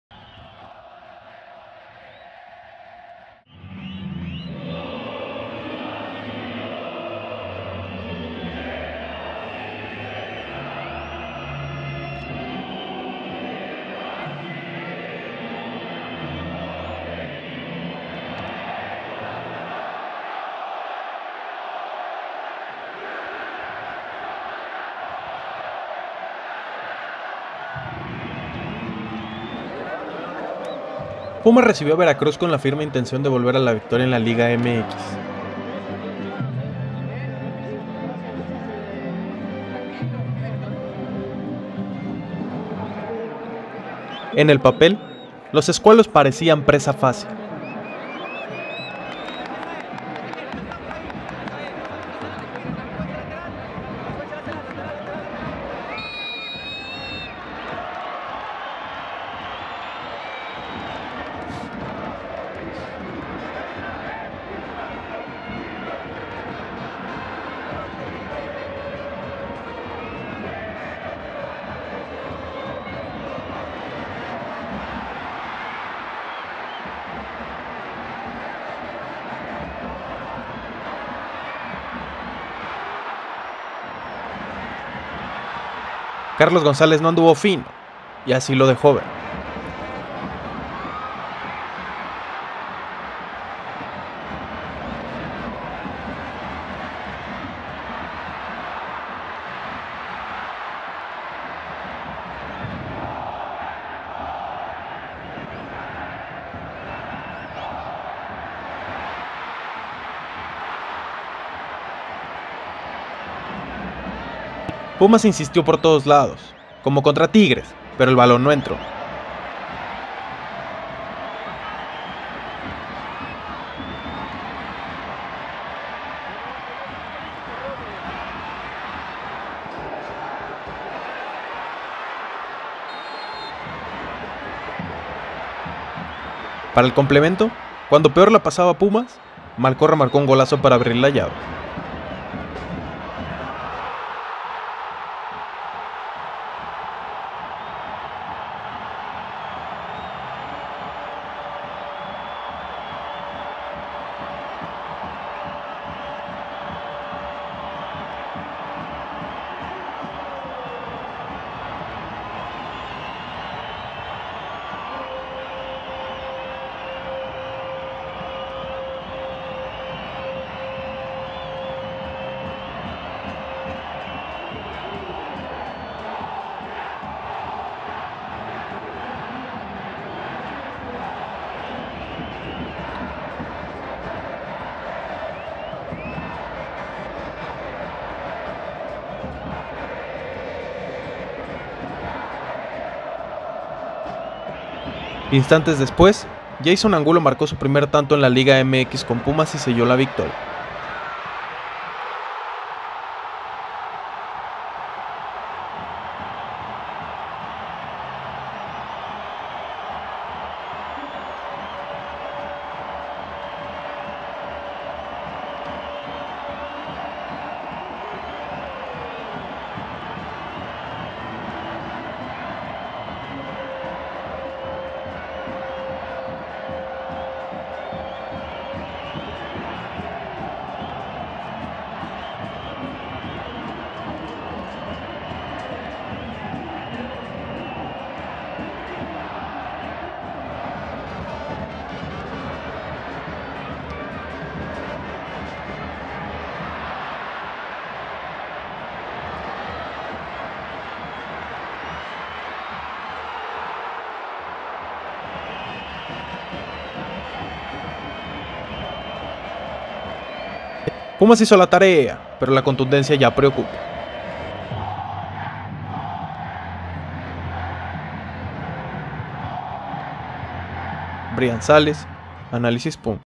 PUMAS 2-0 VERACRUZ | LA TARDE DE JEISON
Así vivimos el Pumas 2-0 Veracruz con los goles de Víctor Malcorra y Jeison Angulo.